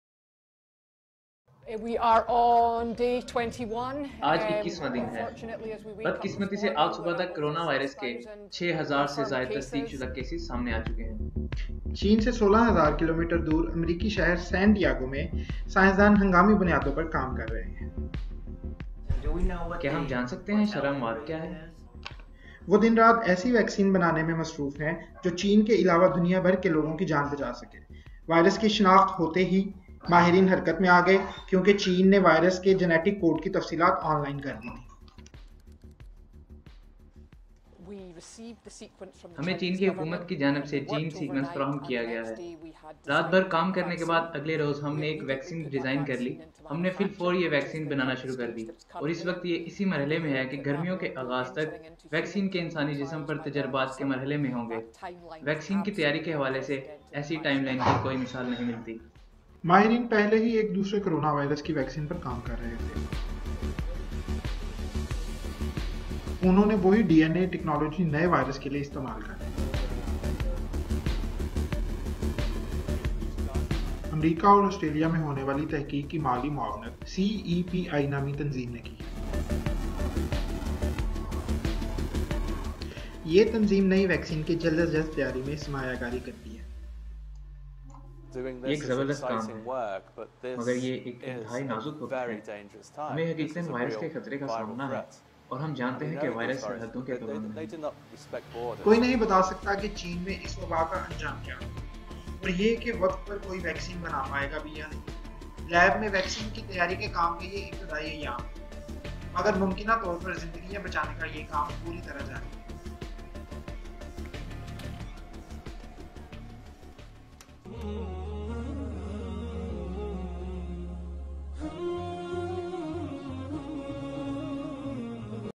Cronavirus treatment vaccine development in USA & Australia کرونا وائرس کے لیے ویکسین کی تیاری شروع
this is video is about the vaccine of  virus spread-ed in  city Wuhan  of china named as Cronavirus. this video shows the developments of vaccine for treatments of virus effected patients.
orignal source of video is bbc:urdu